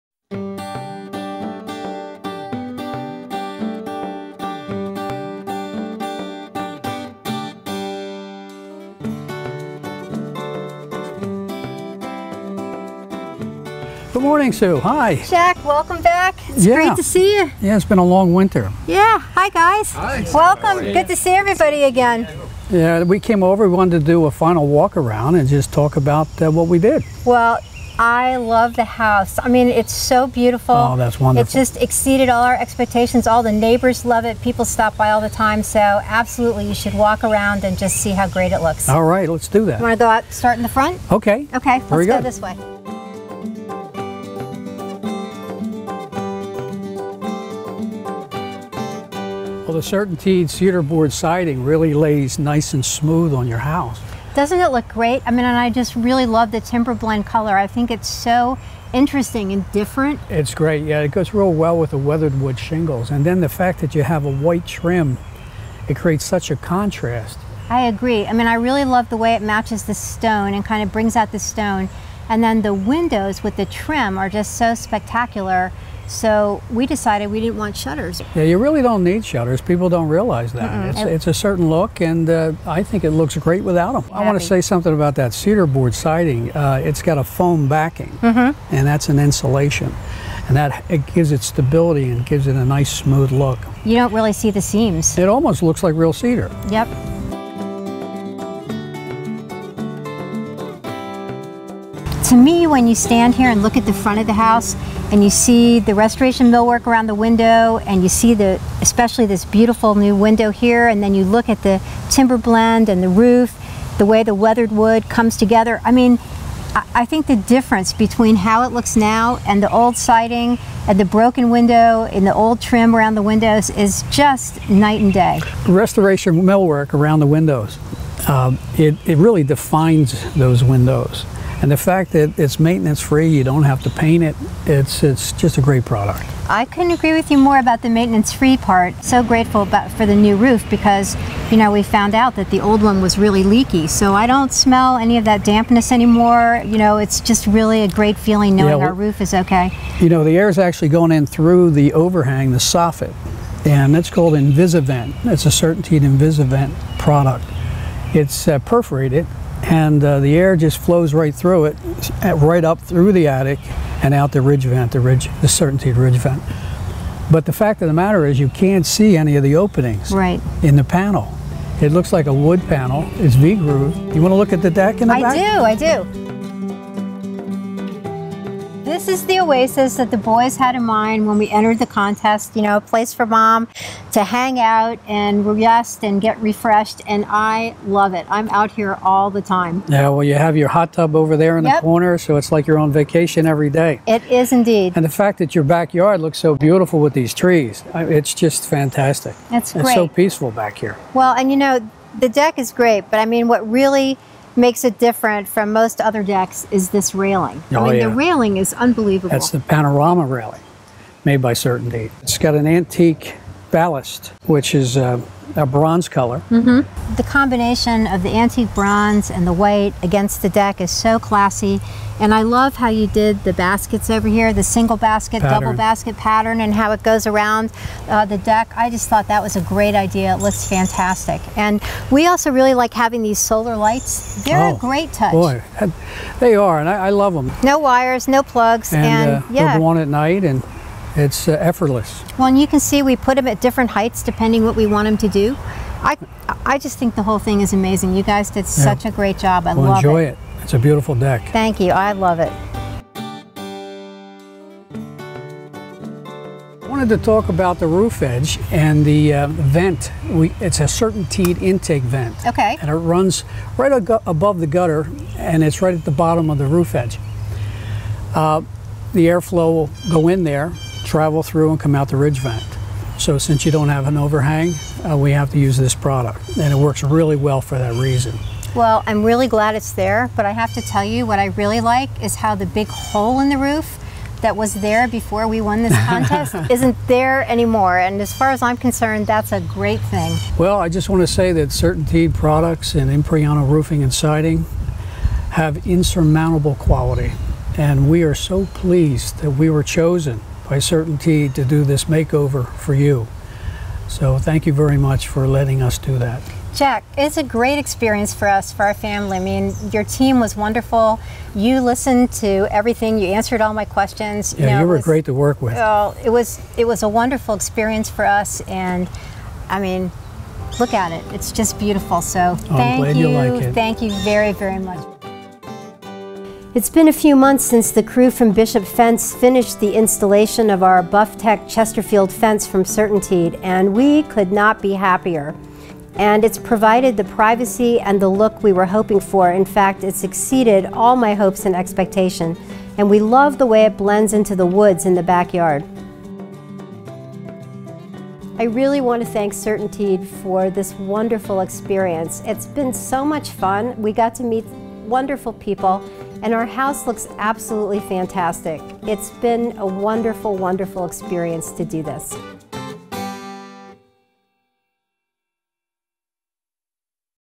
Final Tour of the Berwyn, Pennsylvania CertainTeed Living Spaces™ Exterior Home Makeover
Jack from Impriano Roofing and Sue take a final walk around the 2014 Living Spaces® Exterior Home Makeover, and discuss the CertainTeed products that have transformed her home into her dreamed of backyard oasis.